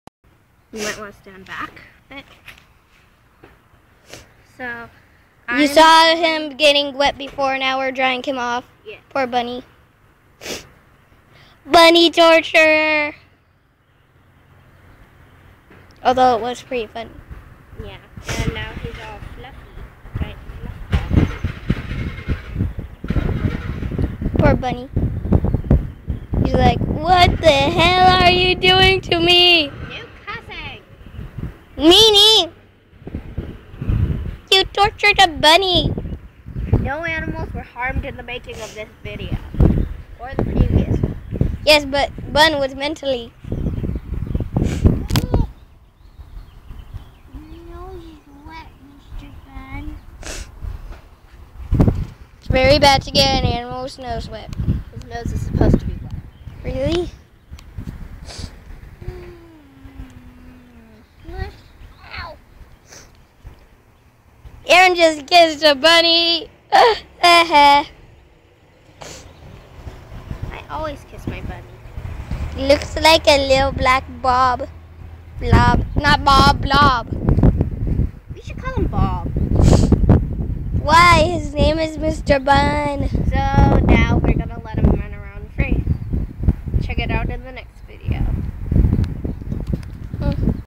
Bunny gets dried off
We had to dry off Mr. Bun after he was sprayed so he wouldn't get sick...

No animals were harmed in the making of this video and no humans were actually sexually harrased by my rabbit.